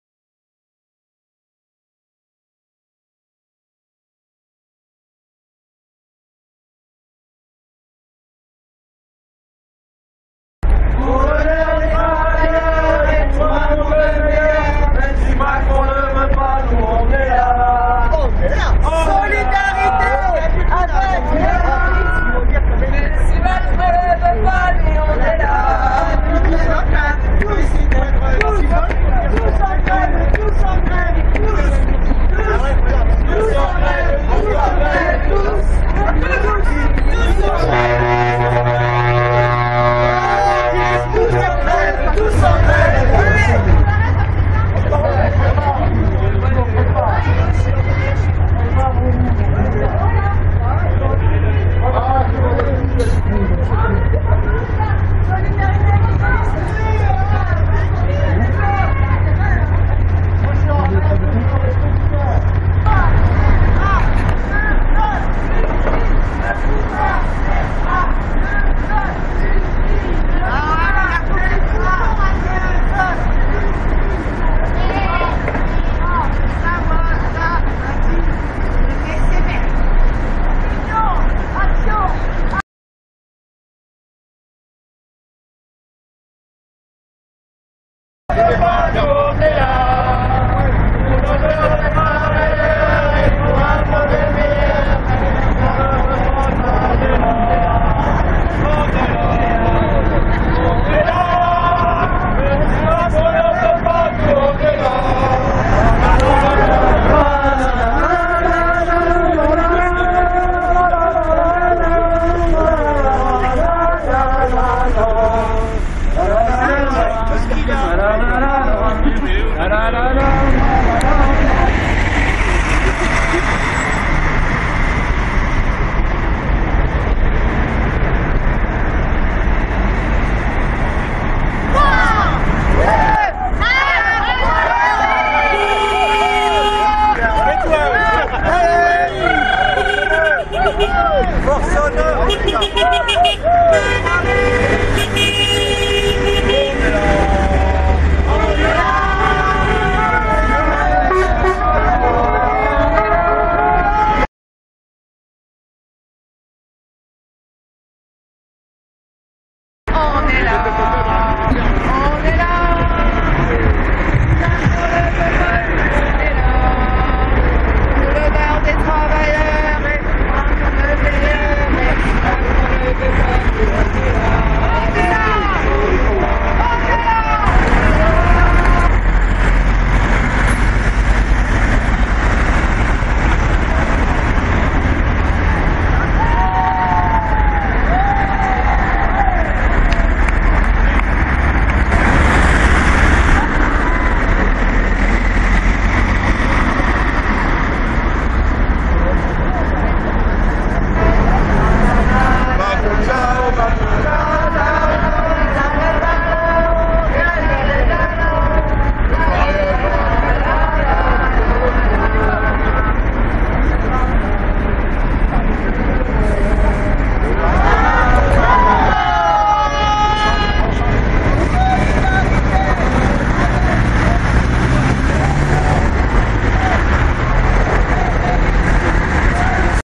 Gilets jaunes - Auxerre 89 Yonne - Swarming (blocage intermittent) à Auxerre. 5 décembre 2019
Gilets jaunes - Auxerre 89 Yonne - Swarming (blocage intermittent) à Auxerre, 89 pour la grève du 5 décembre 2019